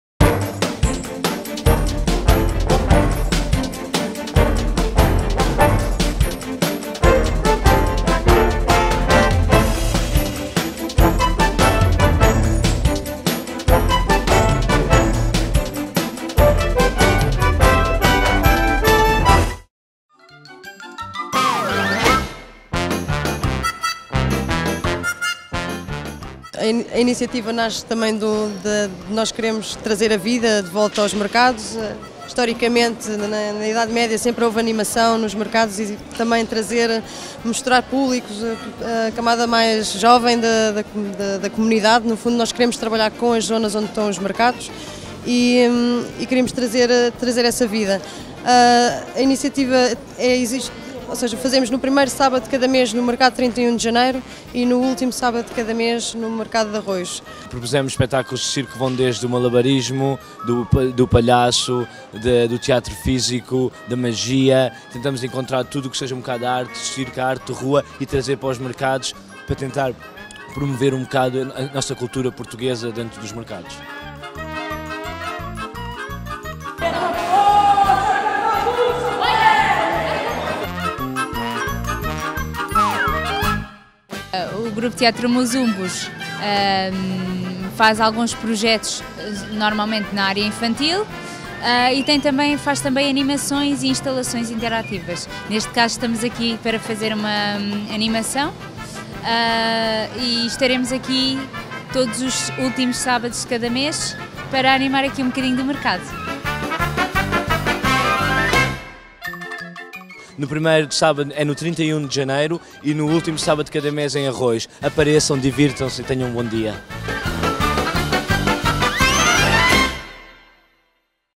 Arroios Informação: Mercado de Arroios | Atividades de Sábado
Amanhã o Mercado de Arroios, situado na Rua Ângela Pinto, faz 75 anos. Para celebrar a data e promover a vivência nestes espaços de comércio tradicional, o mercado esteve em funcionamento durante todo o fim-de-semana, com diversas atividades no âmbito da iniciativa “Circo nos Mercados” e do programa “Artes e Manhas Portuguesas”.